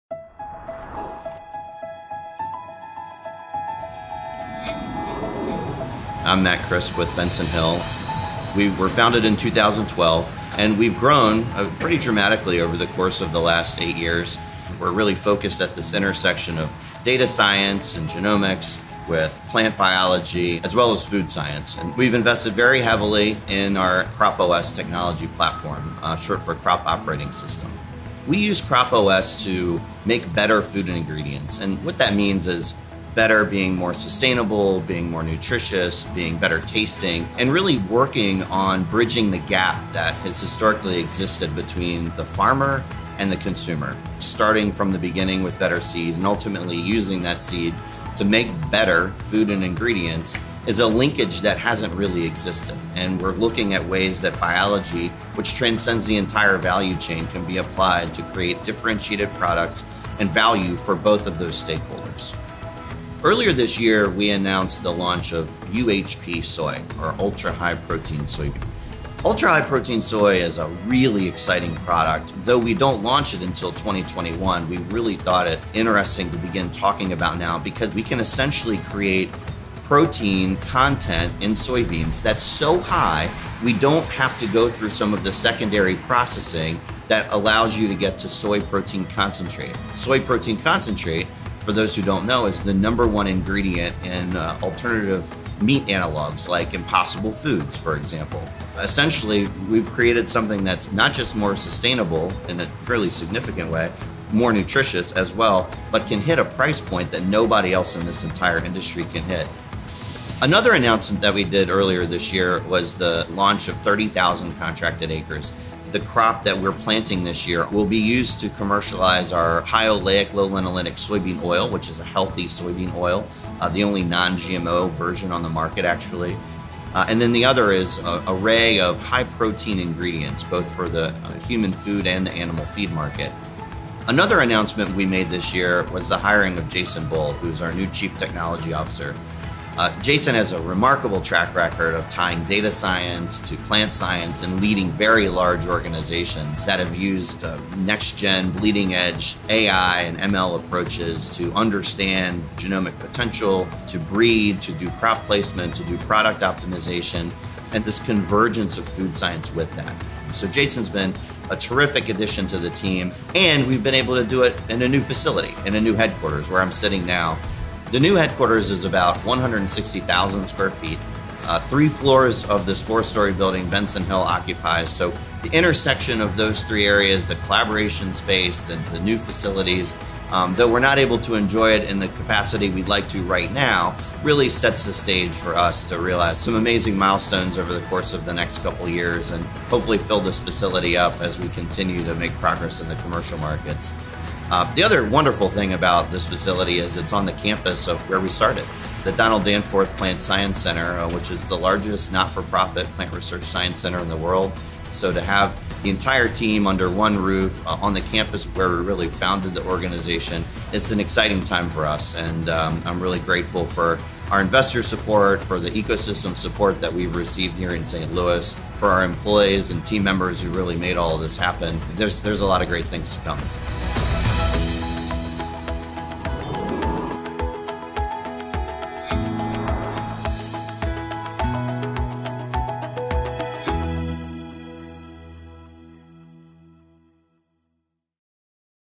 Benson Hill Raises $150M to move food forward.
Benson Hill has announced the close of a $150 million Series D funding round led by Wheatsheaf Group and GV (formerly Google Ventures). The company’s leading food innovation engine CropOS™ taps nature’s genetic diversity to develop and commercialize healthier and more sustainable food and ingredient options that benefit farmers, food companies of all kinds, and ultimately, consumers. This funding round will accelerate the food tech company’s efforts to deploy its platform technology, expand partner development across the supply chain, and scale product commercialization efforts.